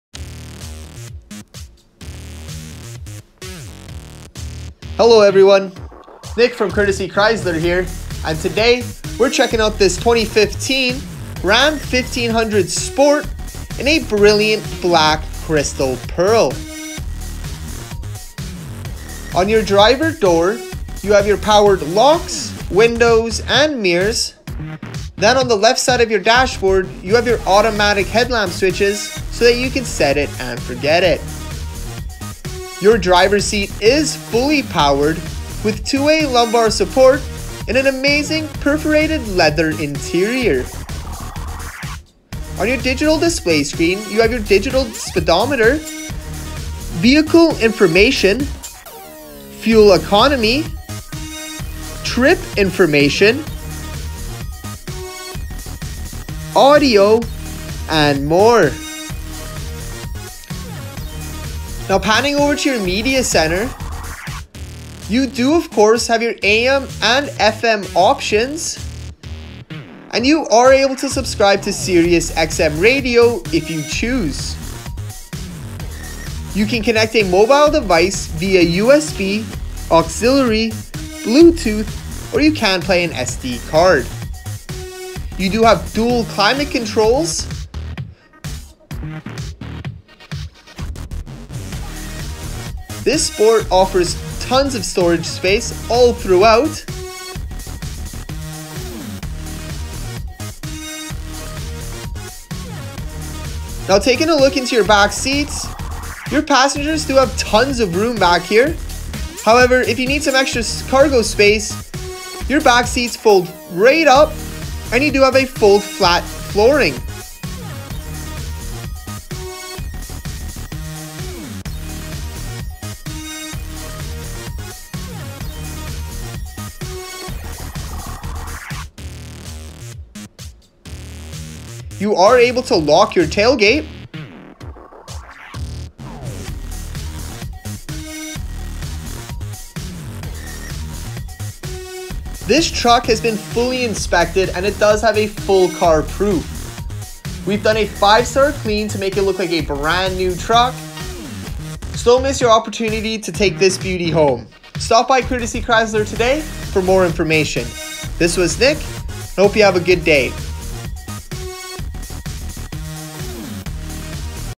2015 Ram 1500 Sport | Brilliant Black Crystal Pearl | Courtesy Chrysler
For more information about this vehicle please visit our

Take a look at this 2015 Ram 1500 Sport! This V6 8-speed transmission is great on fuel and comes with only 63,000kms! Expect nothing short of a smooth drive and take command of the winter road conditions in the capable truck. Features include: 8.4" touch screen with Bluetooth to allow you to stream your favorite music!

Created by: Nick Patterson
Stock: 70356A